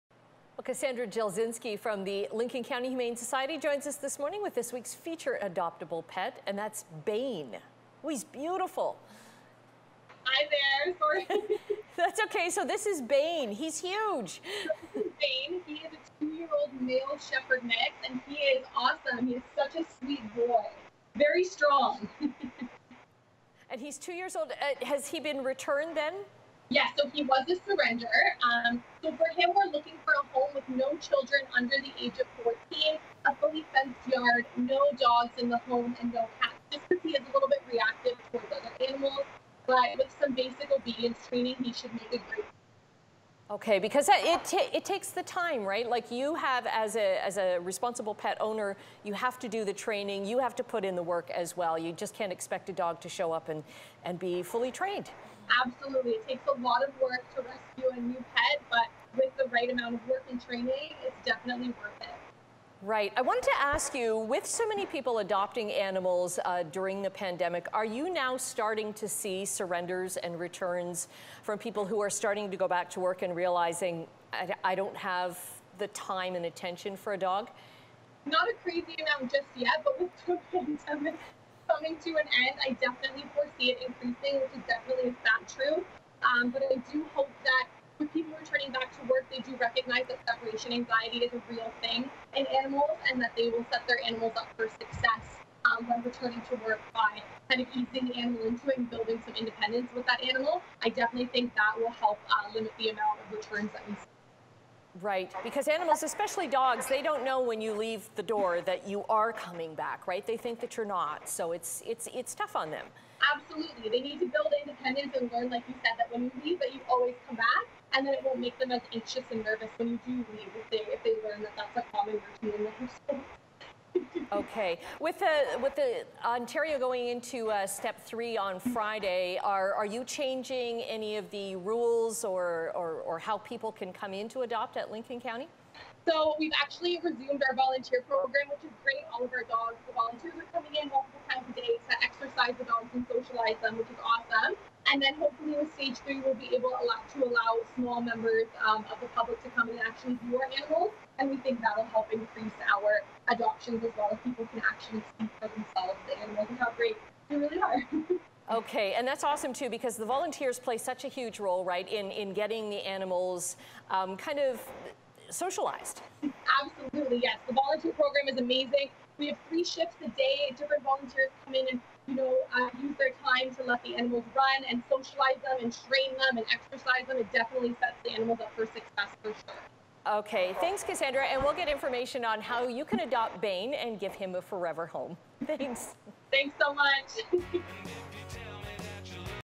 Adoptable Pets: Lincoln County Humane Society
Cassandra Gelzynsky from the Lincoln County Humane Society joined us with Bain, who is this week's featured adoptable pet.